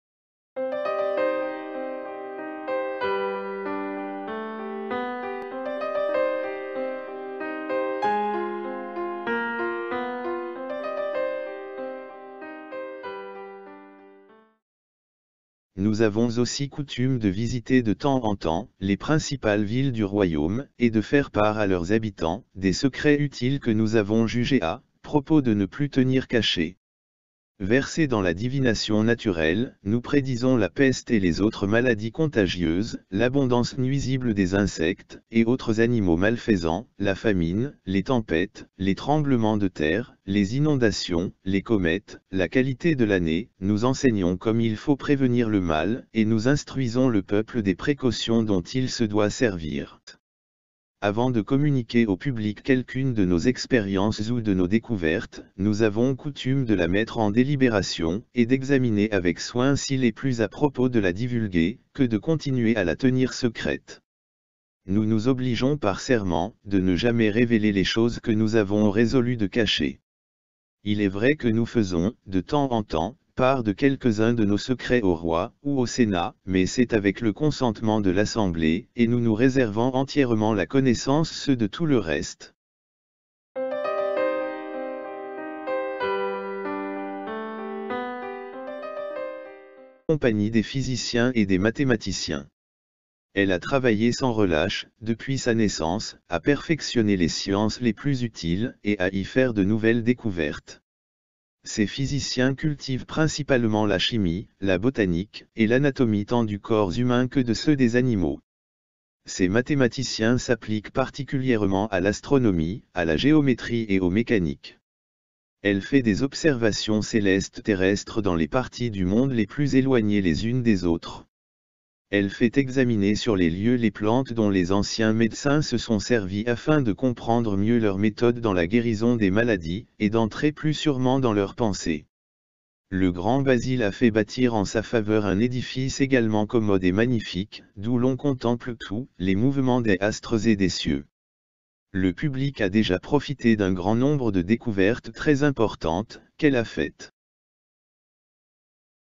Francis Bacon nous décrit l'OMS dans sa Nouvelle Atlantide
Lecture audio tirée de la chaînes Livres ésotériques anciens. Francis Bacon a rédigé cette oeuvre vers 1624.